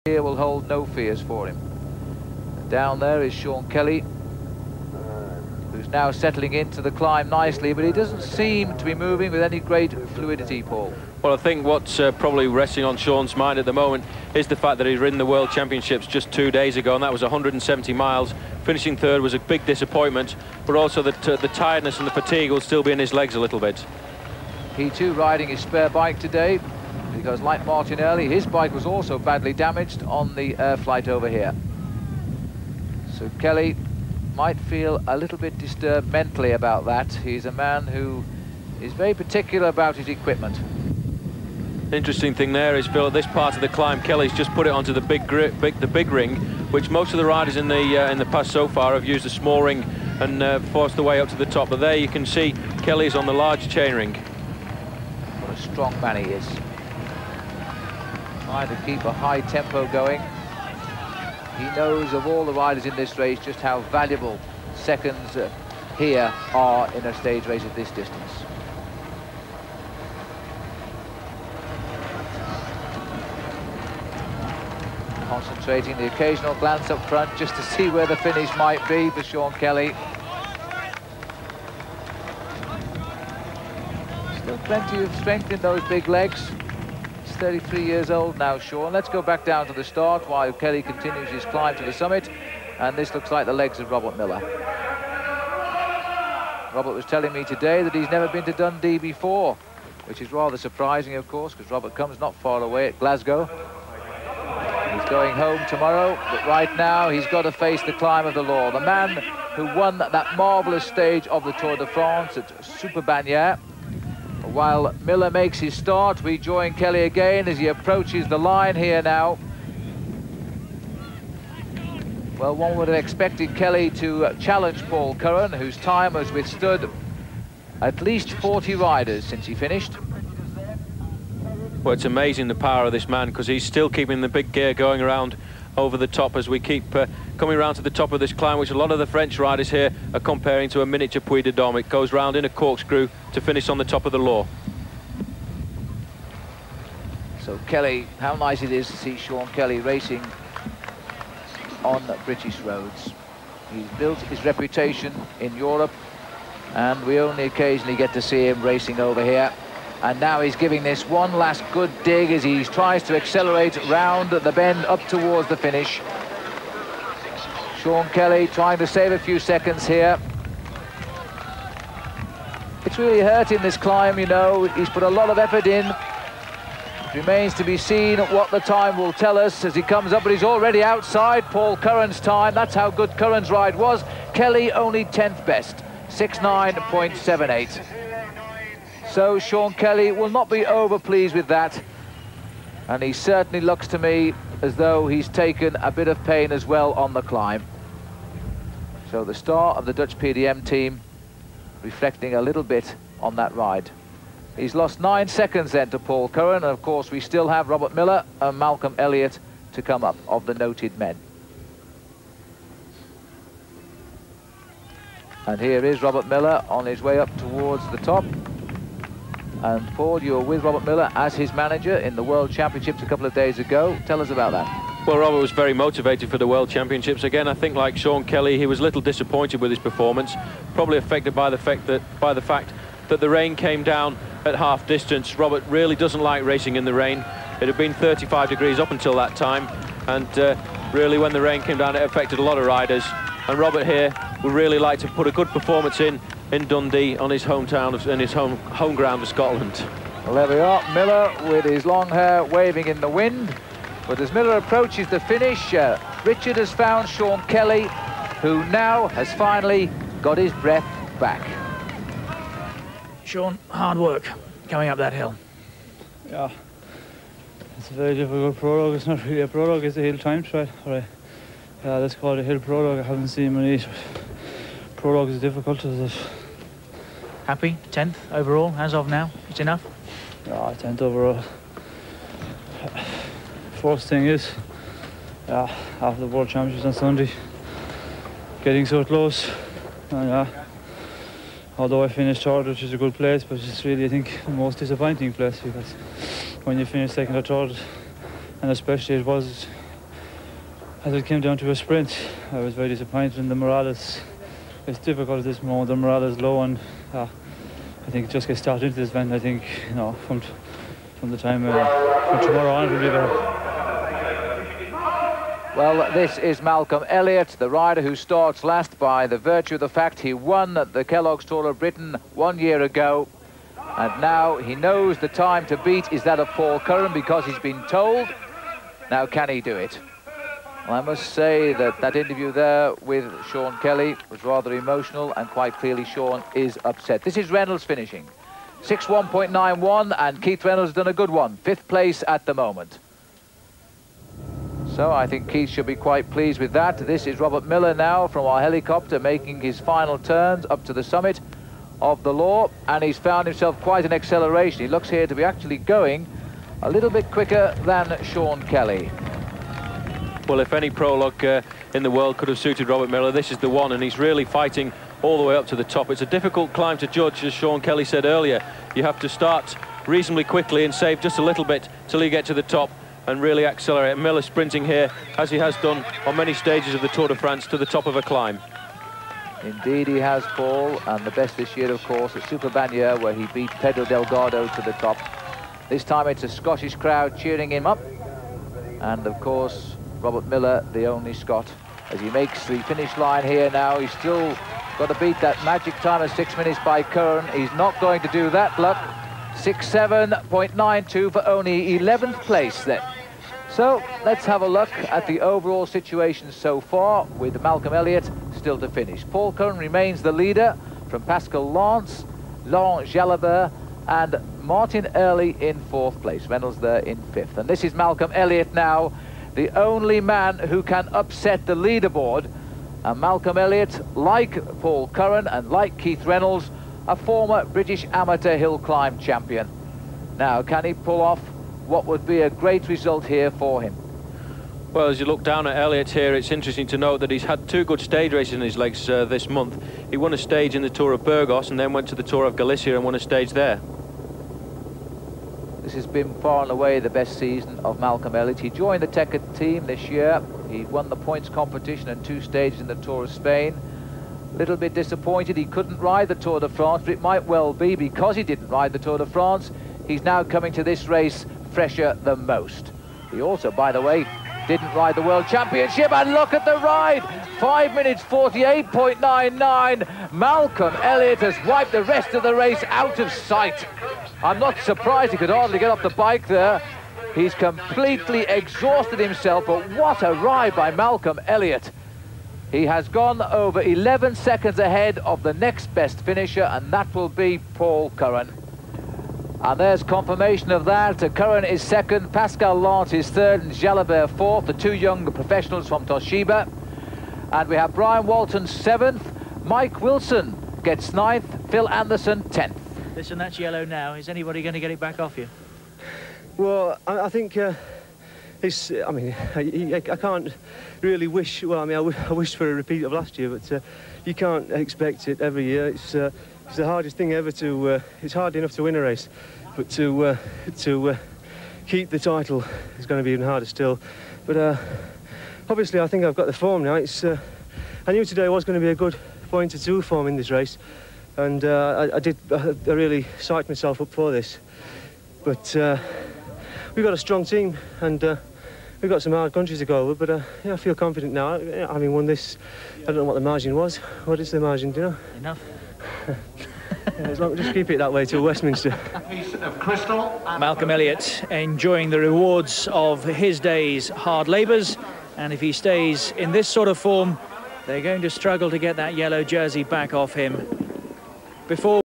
1989 Tour of Britain - Prologue (Part 2)
1989 Kelloggs Tour of Britain Prologue, on The Law in Dundee.